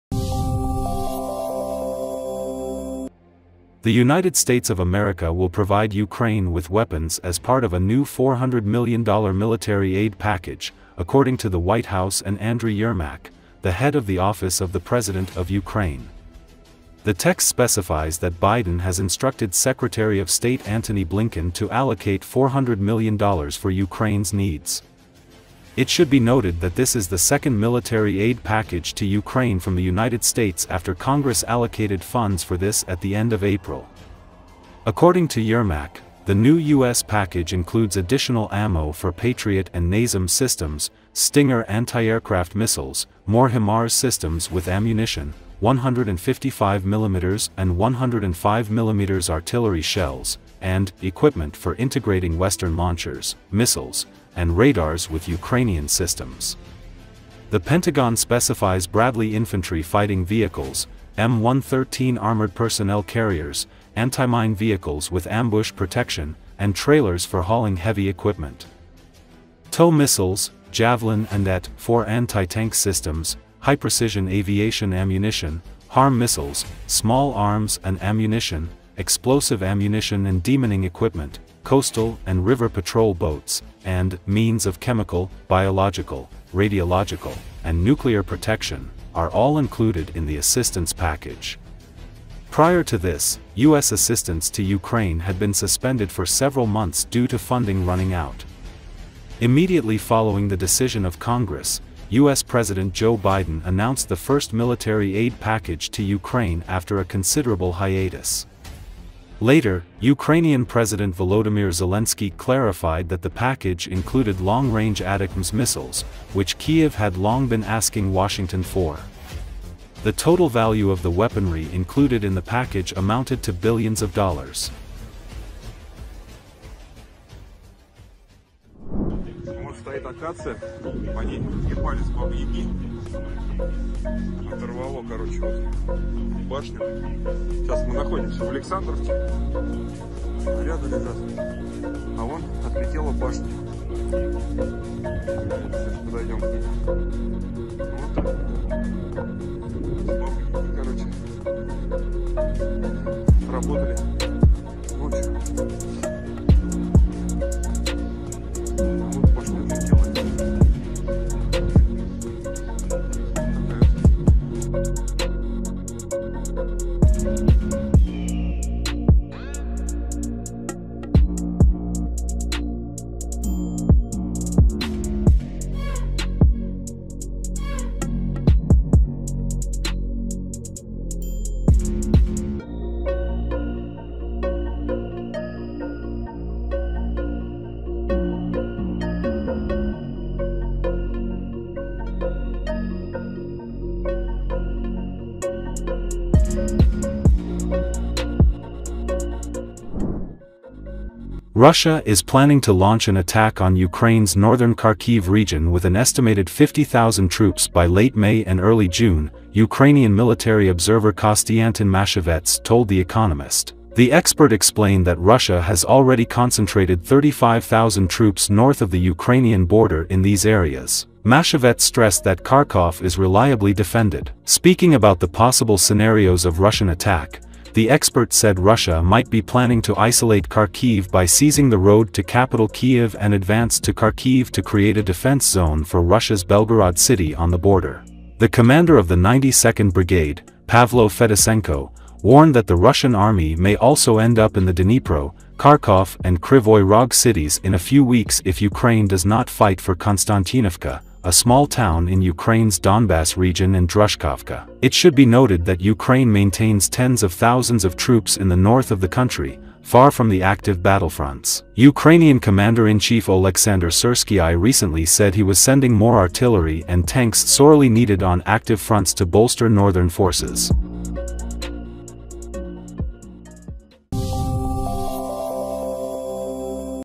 HIMARS, Patriot missiles and more: US sends new military aid package to Ukraine
The United States of America will provide Ukraine with weapons as part of a new $400 million military aid package, according to the White House and Andrii Yermak, the head of the Office of the President of Ukraine.
The text specifies that Biden has instructed Secretary of State Antony Blinken to allocate $400 million for Ukraine's needs.
It should be noted that this is the second military aid package to Ukraine from the United States after Congress allocated funds for this at the end of April.
According to Yermak, the new US package includes additional ammo for Patriot and NASAMS systems, Stinger anti-aircraft missiles, more HIMARS systems with ammunition, 155-mm and 105-mm artillery shells, and "equipment for integrating Western launchers, missiles, and radars with Ukrainian systems".
The Pentagon specifies Bradley infantry fighting vehicles, M113 armoured personnel carriers, anti-mine vehicles with ambush protection, and trailers for hauling heavy equipment.
TOW missiles, Javelin and AT-4 anti-tank systems, "high-precision aviation ammunition", HARM missiles, small arms and ammunition, explosive ammunition and demining equipment, coastal and river patrol boats, and "means of chemical, biological, radiological, and nuclear protection" are all included in the assistance package.
Prior to this, US assistance to Ukraine had been suspended for several months due to funding running out.
Immediately following the decision of Congress, US President Joe Biden announced the first military aid package to Ukraine after a considerable hiatus. Later, Ukrainian President Volodymyr Zelenskyy clarified that the package included long-range ATACMS missiles, which Kyiv had long been asking Washington for.
The total value of the weaponry included in the package amounted to billions of dollars.